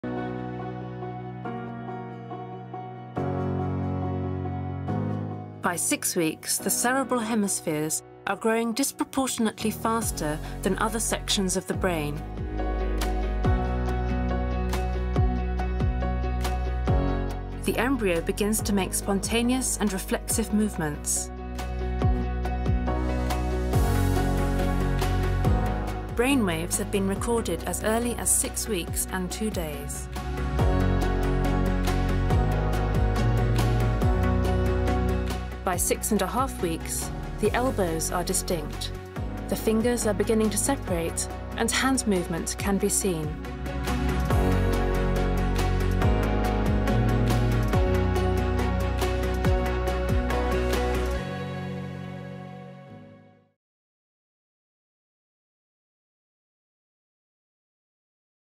This is baby's 6th week after fertilization.
Many of the routine functions performed by the adult body become established during pregnancy – often long before birth.  The developmental period before birth is understood as a time of preparation during which the baby acquires the many structures, and practices the many skills, needed for survival after birth.

The science of embryology shows the unborn are living distinct whole human beings from the moment of fertilization. All humans are Intrinsically valuable, unique, even down to the DNA in their cells.

All embryonic and fetal ages in Baby Life Begins refer to the time since fertilization.

Pregnancy in humans normally lasts approximately 38 weeks as measured from the time of fertilization,  or conception,  until birth.

During the first 8 weeks following fertilization, the baby is called an embryo,  which means “growing within.”  This time, called the embryonic period,  is characterized by the formation of most major body systems.

From the completion of 8 weeks until the end of pregnancy, “the baby is called a fetus,” which means “unborn offspring.” During this time, called the fetal period, the baby grows larger and its systems begin to function.

From fertilization to birth and beyond, human development is dynamic, continuous, and complex. New discoveries about this fascinating process increasingly show the vital impact of fetal development on lifelong health. As our understanding of early human development advances, so too will our ability to enhance health – both before and after birth.

00:00 6th week from fertilization
00:16 Spontaneous movement, brains waves start
01:05 BLB close